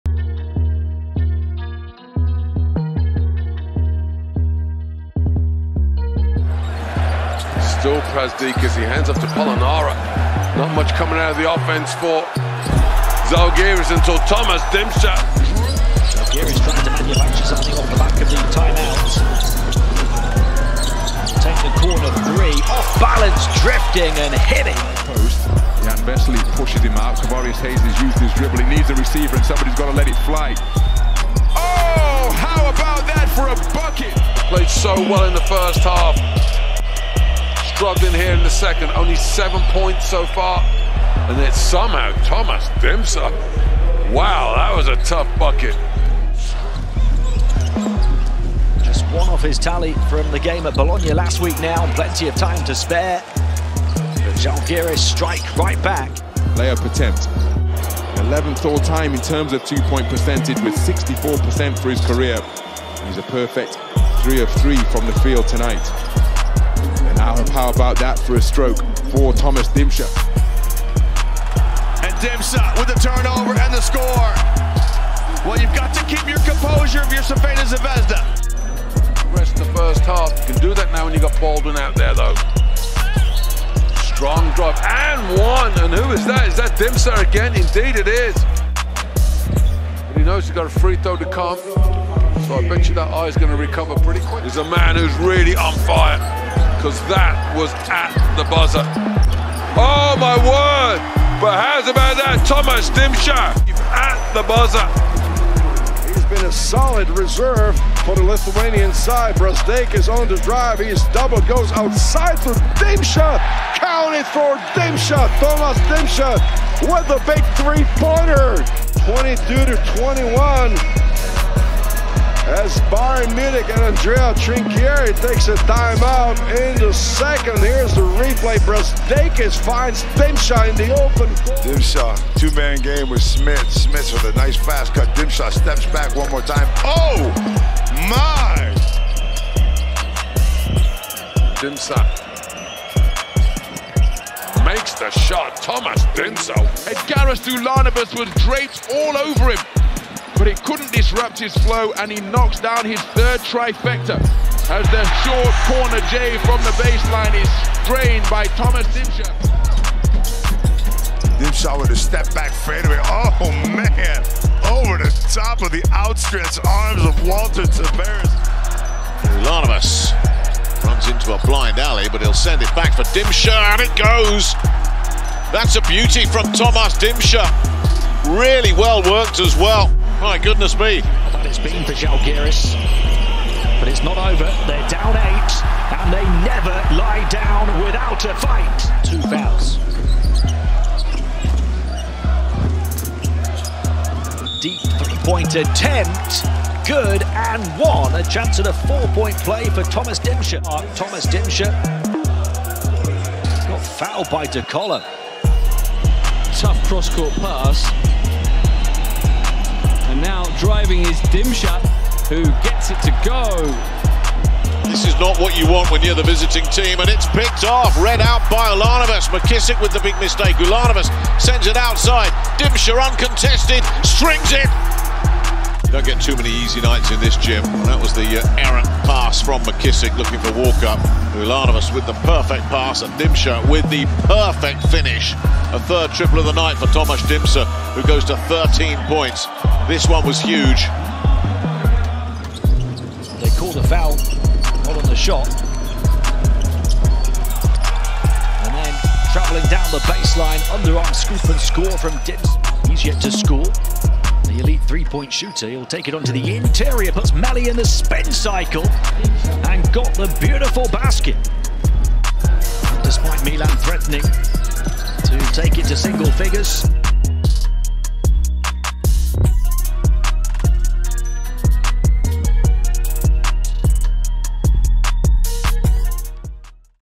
Tomas Dimsa | 2022-2023 season highlights
Check out the highlights of Zalgiris Kaunas player Tomas Dimsa from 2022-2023 EuroLeague season.